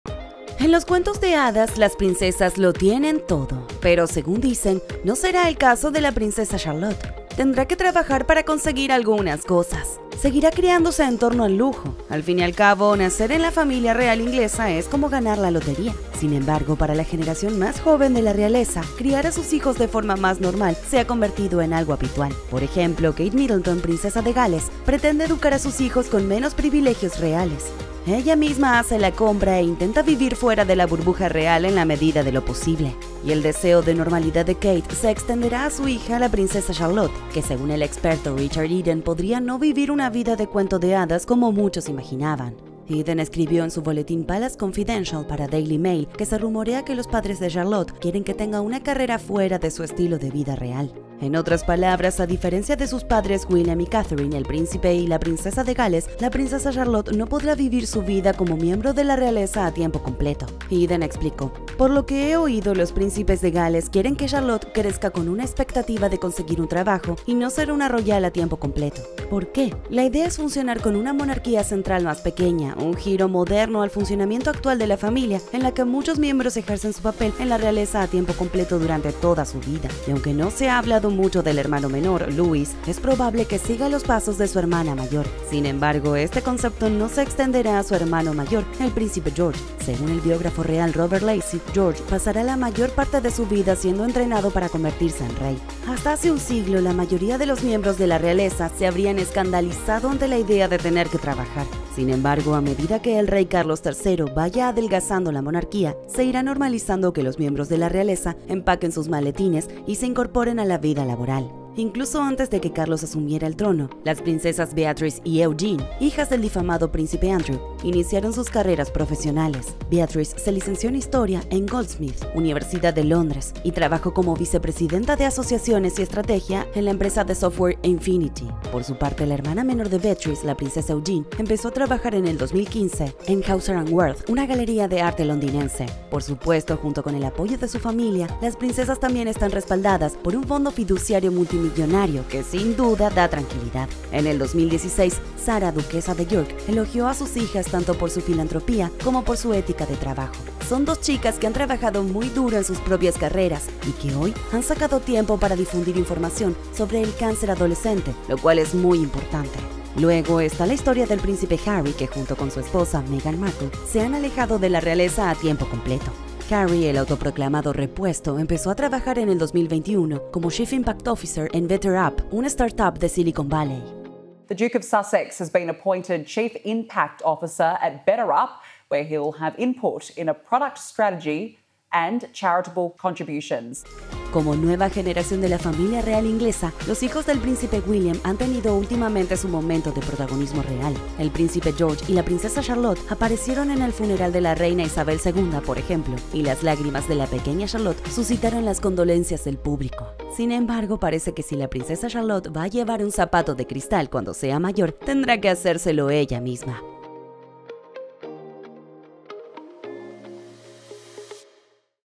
Por Qué La Princesa Charlotte No Será Una Royal Trabajadora
En los cuentos de hadas, las princesas lo tienen todo. Pero, según dicen, no será el caso de la Princesa Charlotte. Tendrá que trabajar para conseguir algunas cosas.

Seguirá criándose en torno al lujo. Al fin y al cabo, nacer en la familia real inglesa es como ganar la lotería. Sin embargo, para la generación más joven de la realeza, criar a sus hijos de forma más normal se ha convertido en algo habitual. Por ejemplo, Kate Middleton, Princesa de Gales, pretende educar a sus hijos con menos privilegios reales. Ella misma hace la compra e intenta vivir fuera de la burbuja real en la medida de lo posible.

Y el deseo de normalidad de Kate se extenderá a su hija, la Princesa Charlotte, que según el experto Richard Eden podría no vivir una vida de cuento de hadas como muchos imaginaban. Eden escribió en su boletín Palace Confidential para Daily Mail que se rumorea que los padres de Charlotte quieren que tenga una carrera fuera de su estilo de vida real. En otras palabras, a diferencia de sus padres William y Catherine, el Príncipe y la Princesa de Gales, la Princesa Charlotte no podrá vivir su vida como miembro de la realeza a tiempo completo. Eden explicó:

"Por lo que he oído, los príncipes de Gales quieren que Charlotte, crezca con la expectativa de conseguir un trabajo y no ser una royal a tiempo completo".

¿Por qué? La idea es funcionar con una monarquía central más pequeña, un giro moderno al funcionamiento actual de la familia — en la que muchos miembros ejercen su papel en la realeza a tiempo completo durante toda su vida.

Y aunque no se ha hablado mucho del hermano menor, Louis, es probable que siga los pasos de su hermana mayor. Sin embargo, este concepto no se extenderá a su hermano mayor, el Príncipe George. Según el biógrafo real Robert Lacey, George pasará la mayor parte de su vida siendo entrenado para convertirse en rey.

Hasta hace un siglo, la mayoría de los miembros de la realeza se habrían escandalizado ante la idea de tener que trabajar. Sin embargo, a medida que el Rey Carlos III vaya adelgazando la monarquía, se irá normalizando que los miembros de la realeza empaqueten sus maletines y se incorporen a la vida laboral.

Incluso antes de que Carlos asumiera el trono, las Princesas Beatrice y Eugenie, hijas del difamado Príncipe Andrew, iniciaron sus carreras profesionales. Beatrice se licenció en Historia en Goldsmiths, Universidad de Londres, y trabajó como Vicepresidenta de Asociaciones y Estrategia en la empresa de software Afiniti. Por su parte, la hermana menor de Beatrice, la Princesa Eugenie, empezó a trabajar en el 2015 en Hauser & Wirth, una galería de arte londinense.

Por supuesto, junto con el apoyo de su familia, las princesas también están respaldadas por un fondo fiduciario multimillonario, que sin duda da tranquilidad. En el 2016, Sarah, Duquesa de York, elogió a sus hijas tanto por su filantropía como por su ética de trabajo,

"Son dos chicas que han trabajado muy duro en sus propias carreras y que hoy han sacado tiempo para difundir información sobre el cáncer adolescente, lo cual es muy importante".

Luego, está la historia del Príncipe Harry, que junto con su esposa Meghan Markle, se han alejado de la realeza a tiempo completo. Harry, el autoproclamado "repuesto", empezó a trabajar en el 2021 como Chief Impact Officer en BetterUp, una startup de Silicon Valley.

Como nueva generación de la familia real inglesa, los hijos del Príncipe William han tenido últimamente su momento de protagonismo real. El Príncipe George y la Princesa Charlotte aparecieron en el funeral de la Reina Isabel II, por ejemplo, y las lágrimas de la pequeña Charlotte suscitaron las condolencias del público.

Sin embargo, parece que si la Princesa Charlotte va a llevar un zapato de cristal cuando sea mayor, tendrá que hacérselo ella misma.